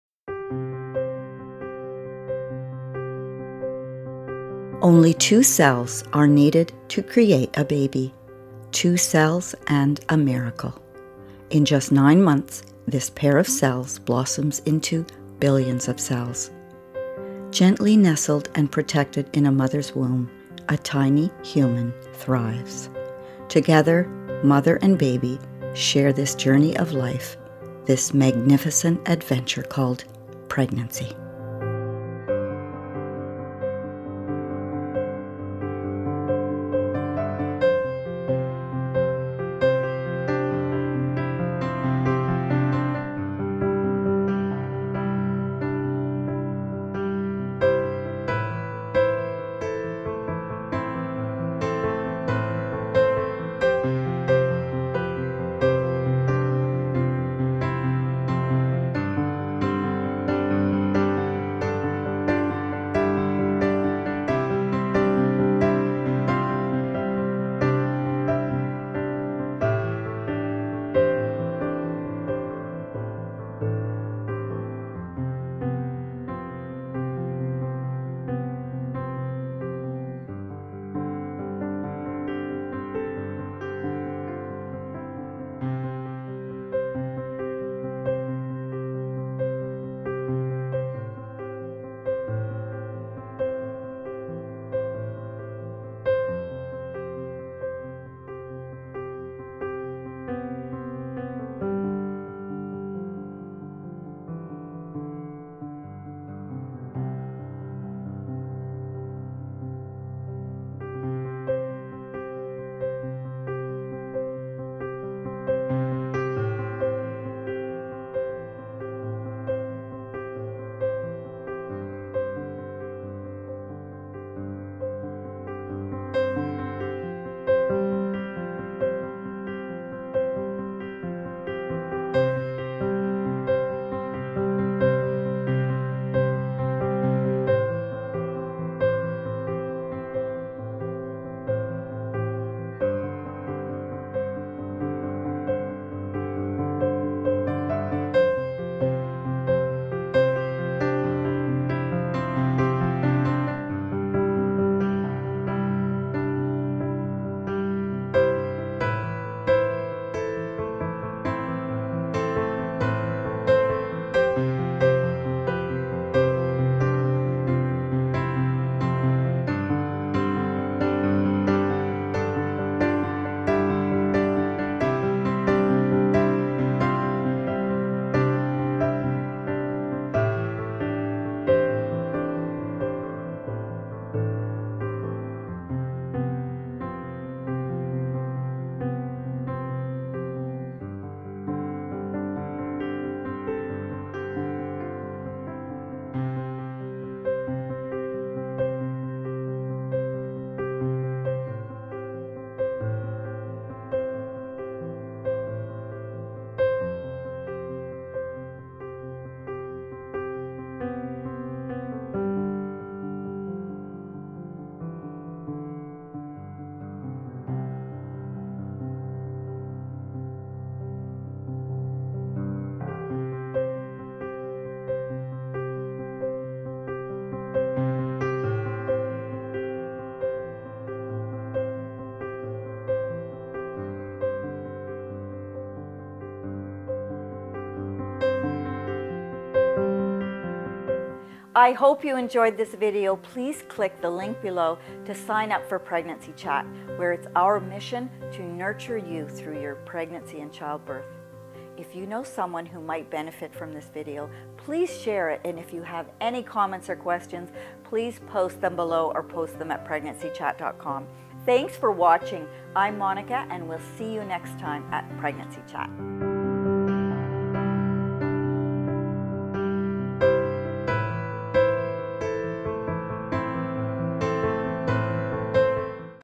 9 Months In The Womb: A Remarkable Look At Fetal Development Through Ultrasound By PregnancyChat.com
9 Months In The Womb: A Remarkable Look At Fetal Development Through Ultrasound By PregnancyChat.Com - Get Pregnancy Updates

From conception to birth, take a unique look at fetal transformation during nine months in the womb. Created by Ultrasound Dimensions and PregnancyChat.com

9 Months In The Womb